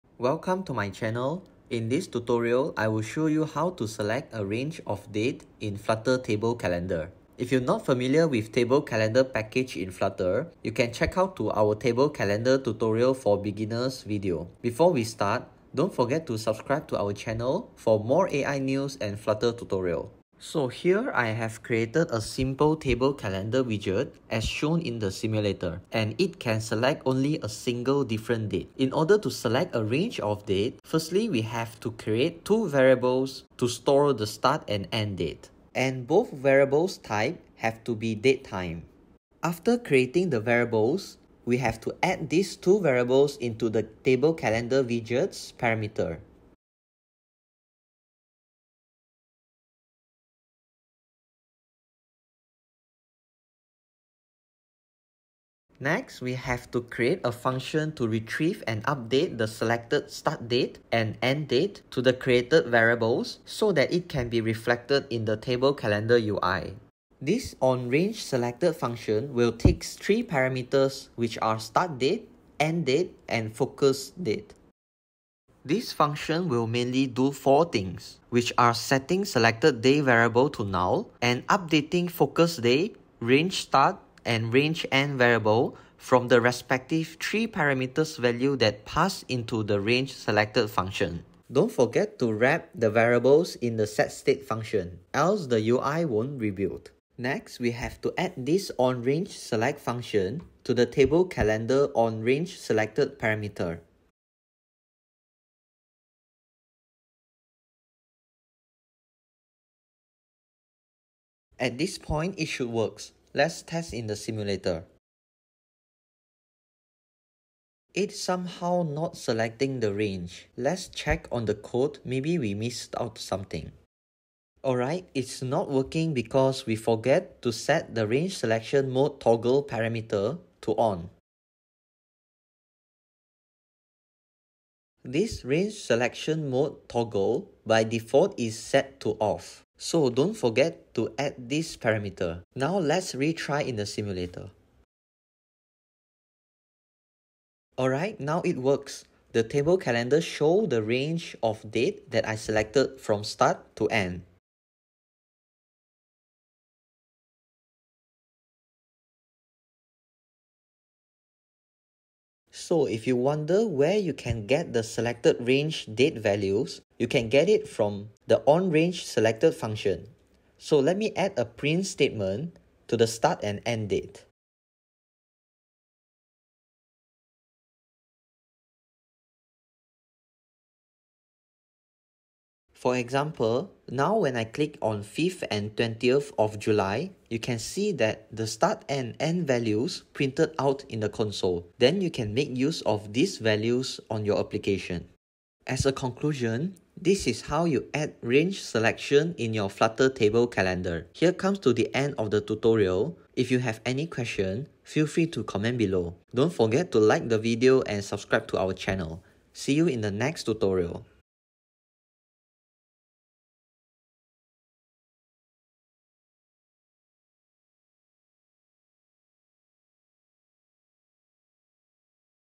How to select a Date Range In Table Calendar Flutter
This tutorial shows you how to pick range of dates in table calendar in your flutter application. For more information about the table calendar flutter package, please visit pub.dev.com for details. Dates range in flutter. Date range in flutter app.